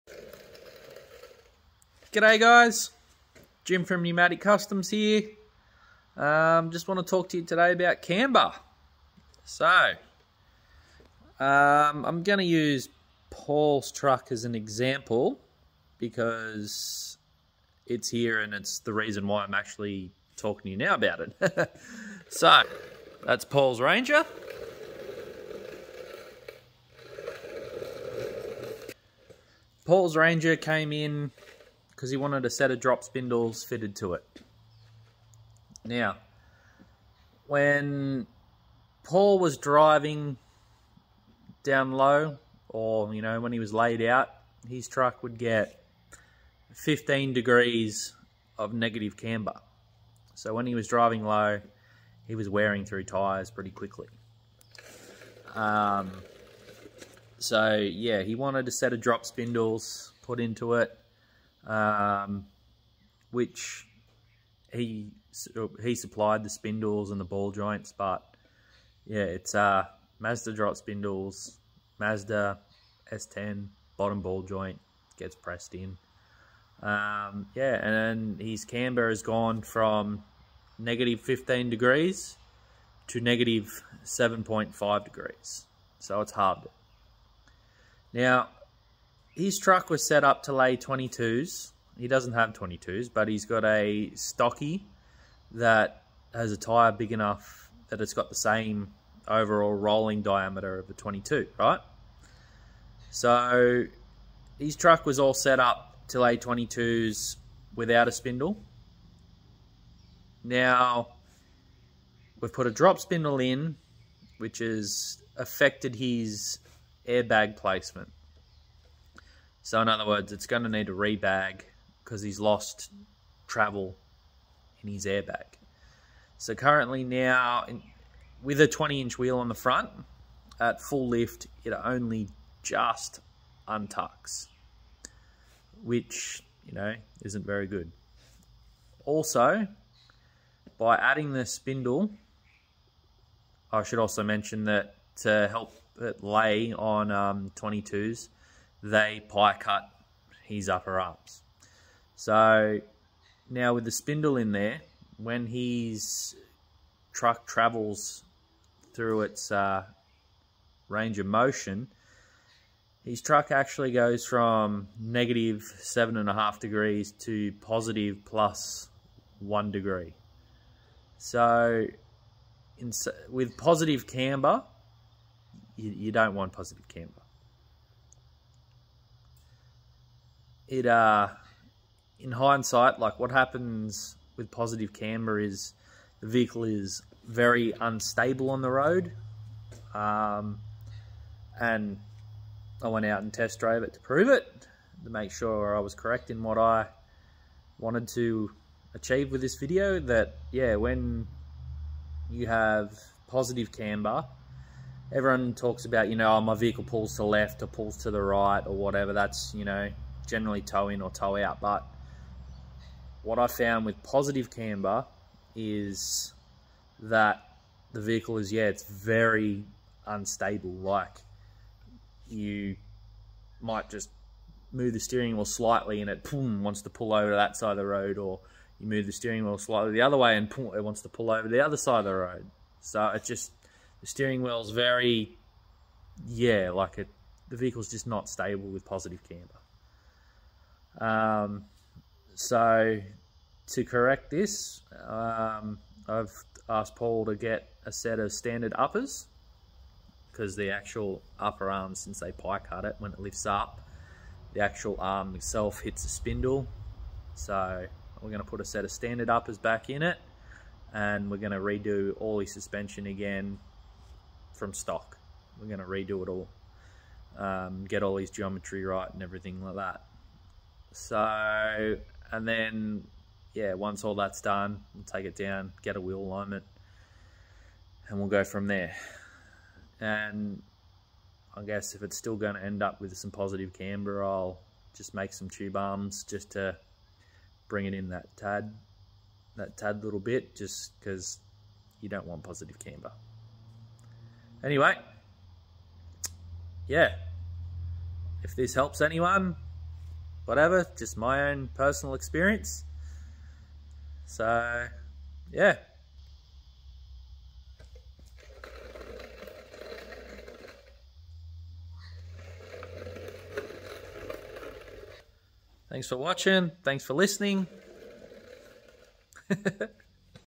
Camber
fitted drop spindles to Paul's ranger and found it gets positive camber at ride height and feels unstable